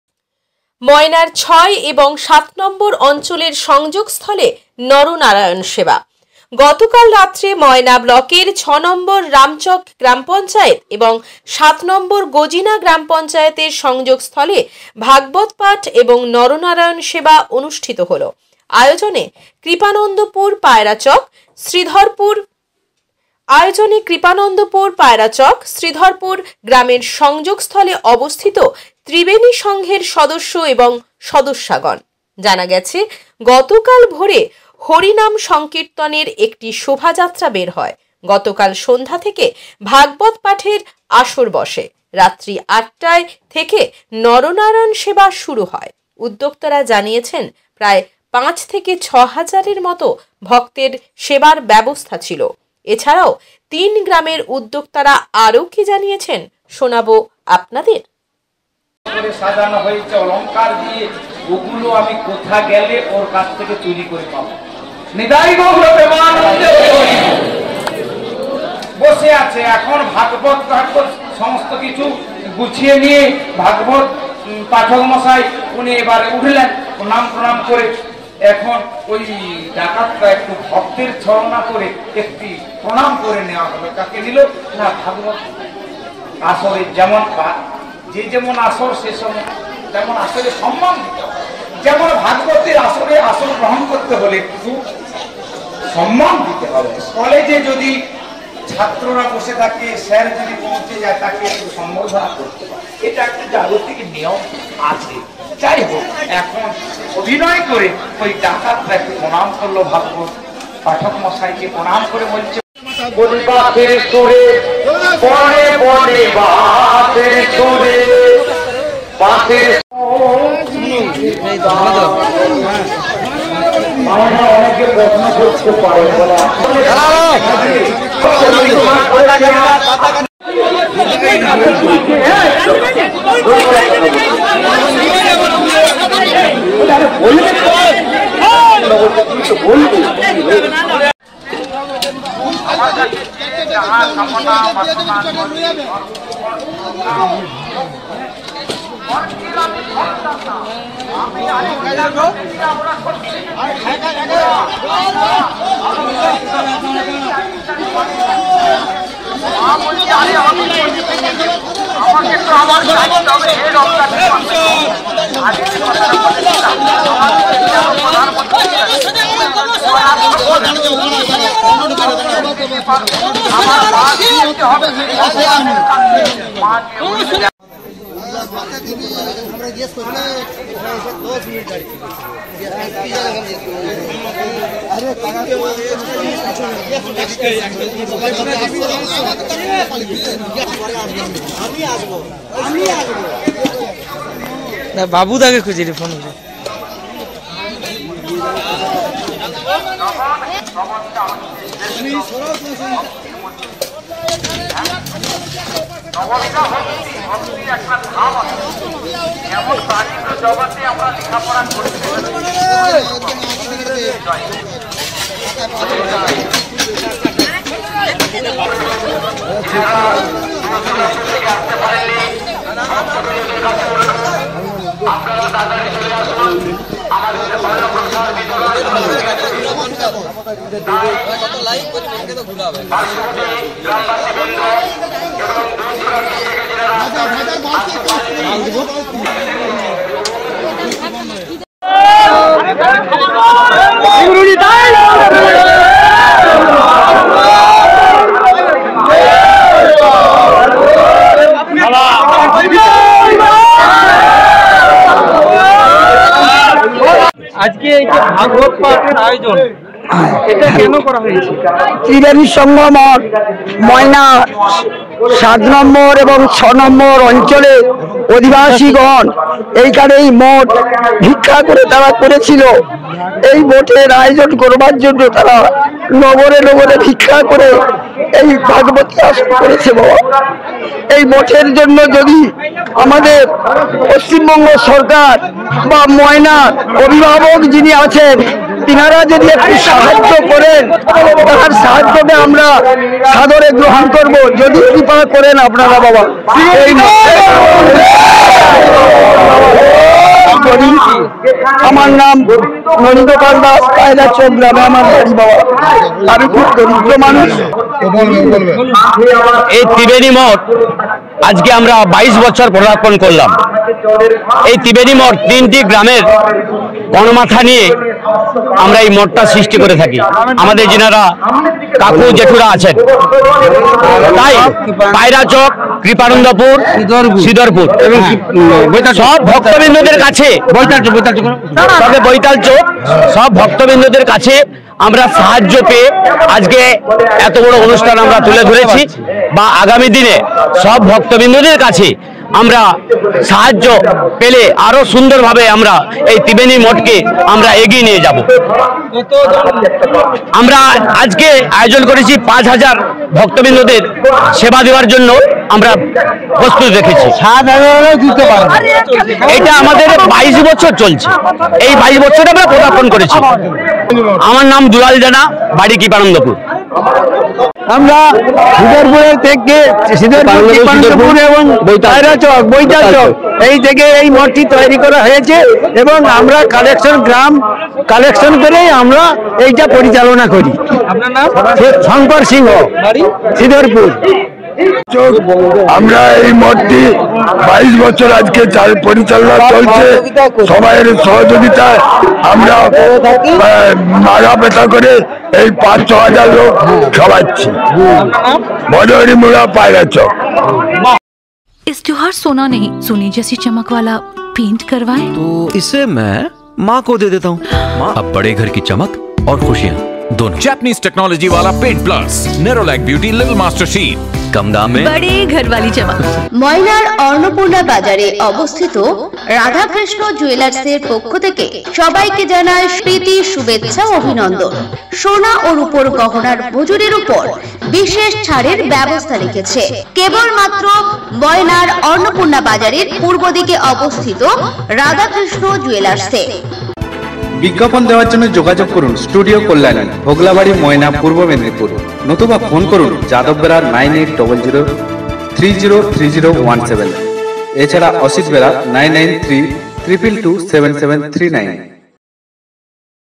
ময়নার ৬ এবং ৭নং অঞ্চলের সংযোগস্থলে নরনারায়ন সেবা
ময়না ব্লকের  ৬ নম্বর রামচক গ্রাম পঞ্চায়েত এবং ৭ নম্বর গোজিনা গ্রাম পঞ্চায়েতের সংযোগস্থলে ভাগবত পাঠ এবং নরনারায়ণ সেবা অনুষ্ঠিত হল | আয়োজনে কৃপানন্দপুর, পায়রাচক, শ্রীধরপুর  গ্ৰামের সংযোগ স্থলে অবস্থিত ত্রিবেনী সংঘের সদস্য এবং সদস্যাগণ | জানা গেছে গতকাল ভোরে হরিনাম সংকীর্তন এর একটি শোভাযাত্রা বের হয় | গতকাল সন্ধ্যা থেকে ভাগবত পাঠের আসর বসে | রাত্রি 8 টায় থেকে নরনারায়ণ সেবা শুরু হয় | উদ্যোক্তারা জানিয়েছেন প্রায় ৫-৬ হাজারের মতো ভক্তের সেবার ব্যবস্থা ছিল | এছাড়াও তিন গ্রামের উদ্যোক্তারা আরো কি জানিয়েছেন শোনাবো আপনাদের